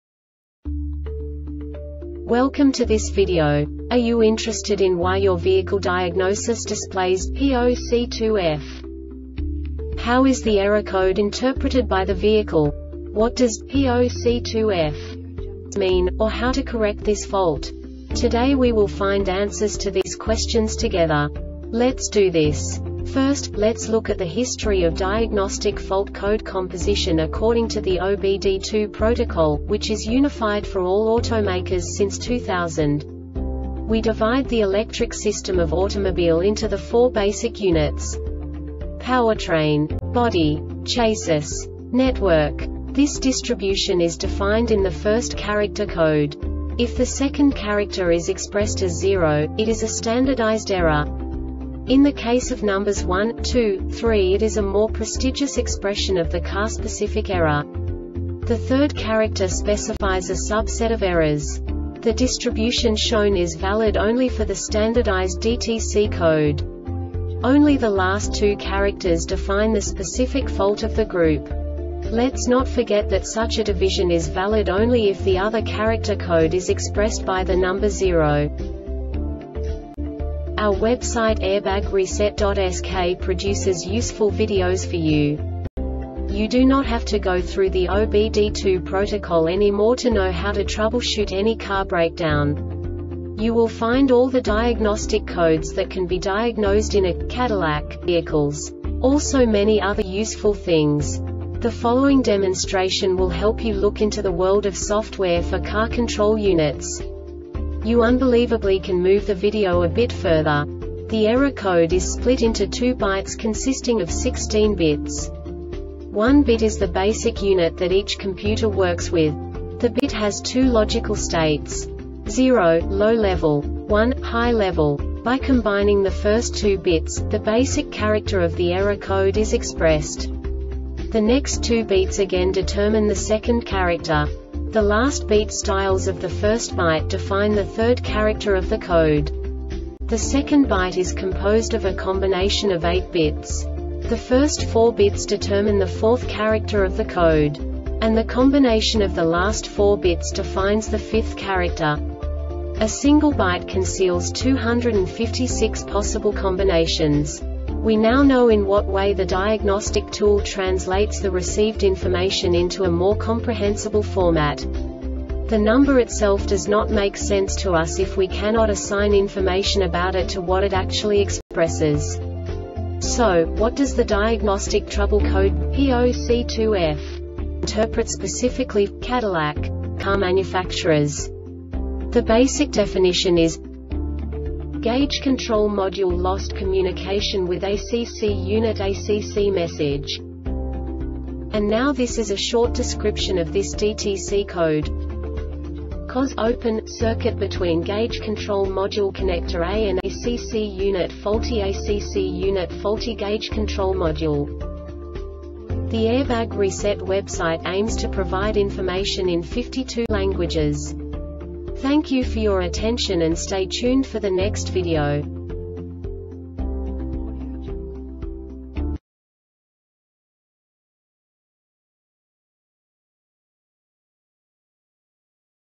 DTC cadillac P0C2F Short Explanation
Contents:
0:21 Basic DTC analysis according to OBD2 protocol standard.
1:48 Insight into programming
2:58 A diagnostically understandable description of the Diagnostic Trouble Code
3:05 Basic error definition

Make:
Cadillac
Code:
P0C2F
Definition:
Gauge Control Module Lost Communication With ACC Unit (ACC message)
Description:
Poor communication between the Gauge Control Module and ACC Unit.NOTE: If you are troubleshooting multiple DTCs,be sure to follow the instructions in B-CAN System Diagnosis Test Mode A.If the HDS does not communicate with the ACC unit,the ACC indicator stay on,and no ACC DTCs are stored,go to symptom troubleshooting for ACC indicator does not go off,and no DTCs are stored.
Cause:
 "Open" circuit between gauge control module connector A and ACC unitFaulty ACC unit Faulty gauge control module